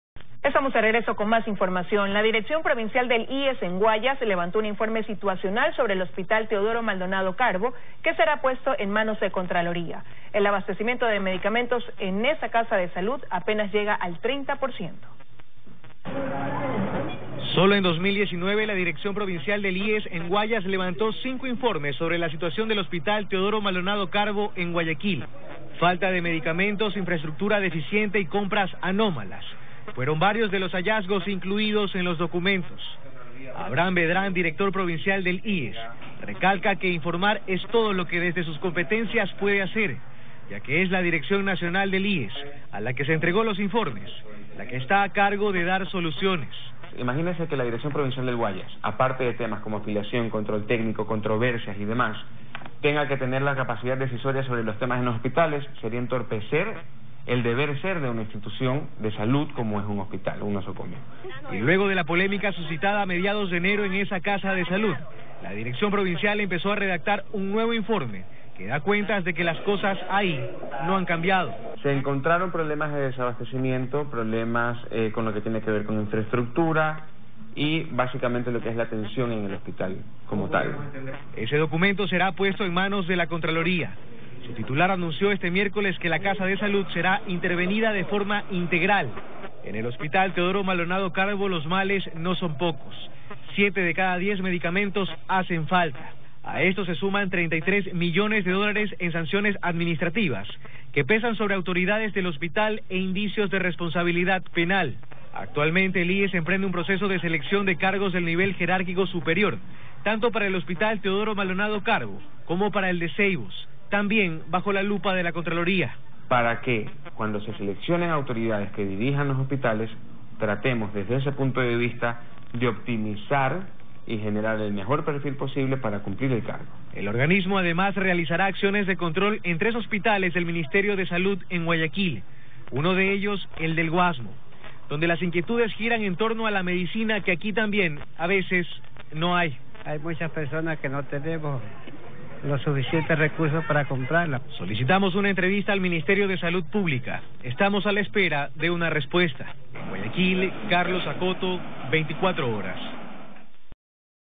DIRECCIÓN PROVINCIAL DEL IESS LEVANTO CINCO INFORMES SOBRE SITUACIÓN DEL HTMC20022020Teleamazonas
DIRECCIÓN PROVINCIAL DEL IESS LEVANTO CINCO INFORMES SOBRE SITUACIÓN DEL HTMC